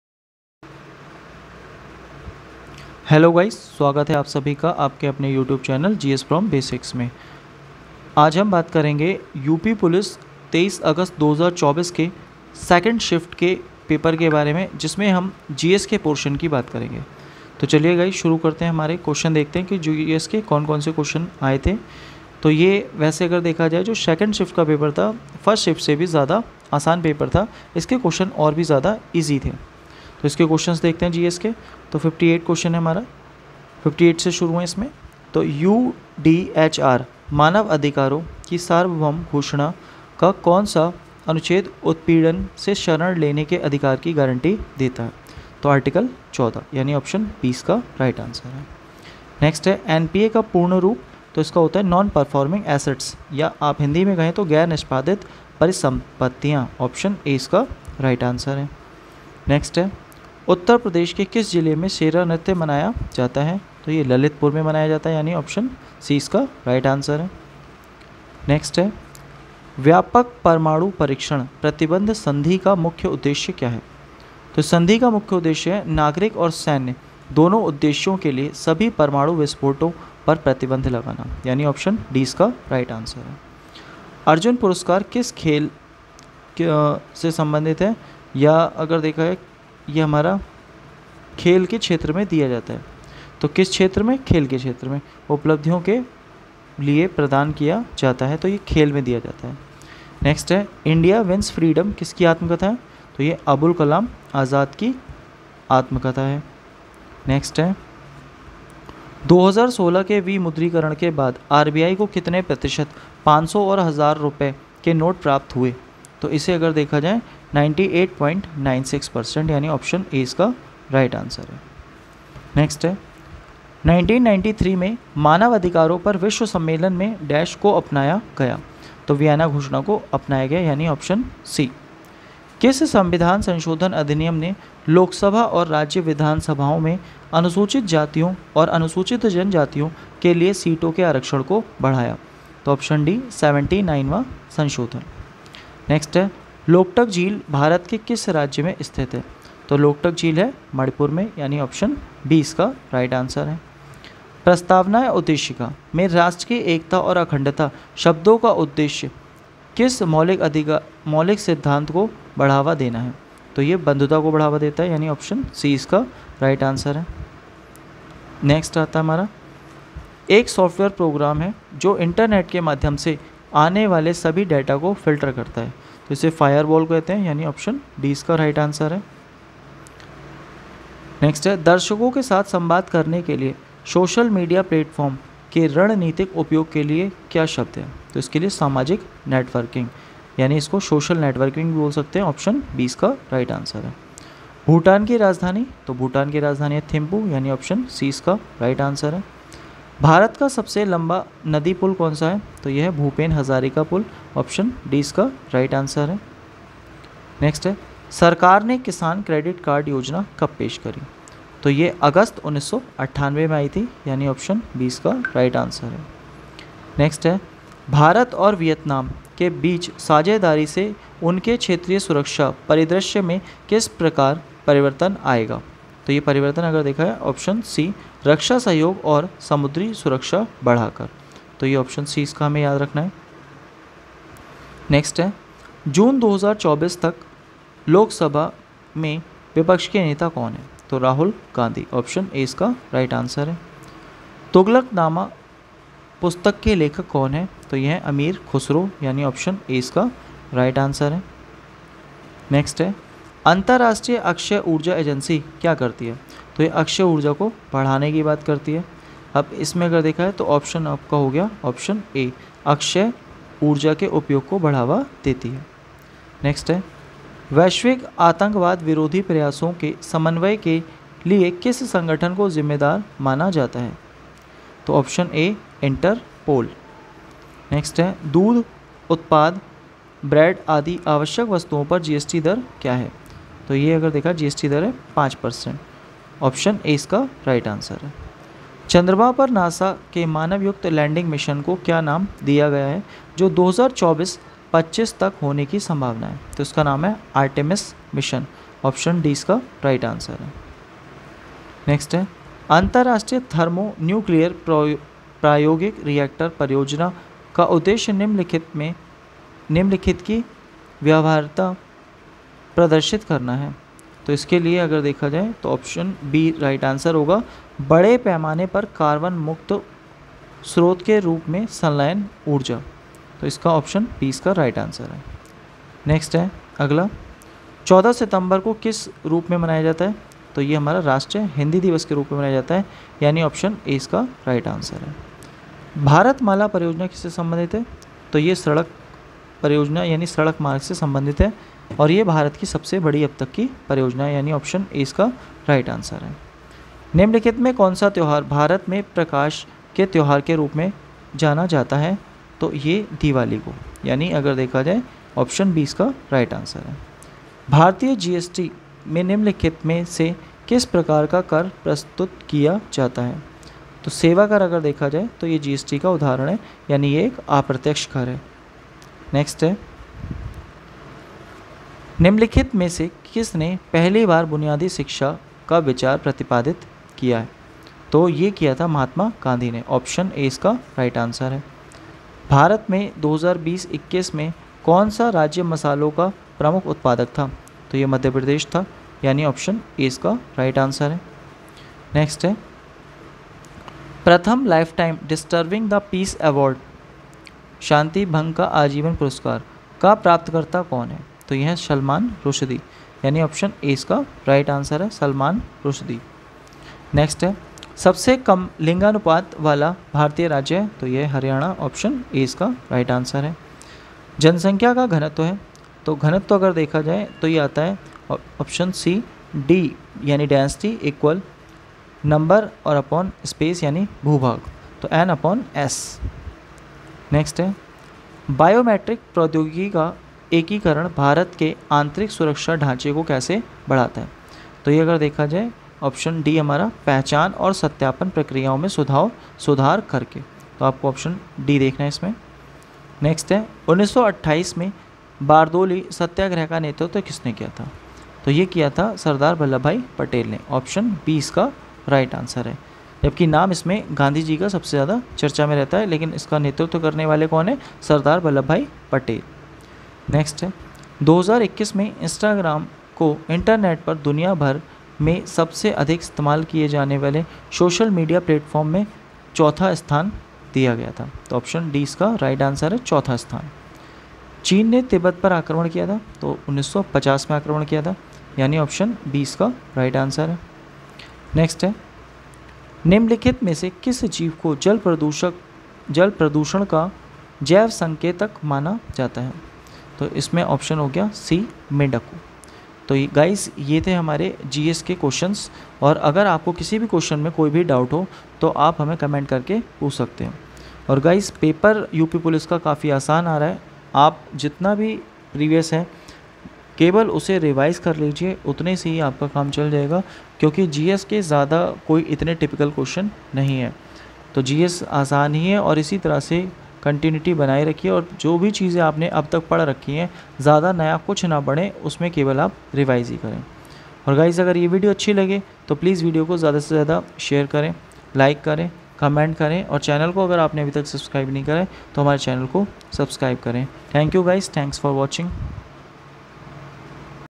23 August 2nd Shift | UPPOLICE RE EXAM GS ANALYSIS | UP Police Answer Key 2024 | Paper Solution 2024
Gs from Basics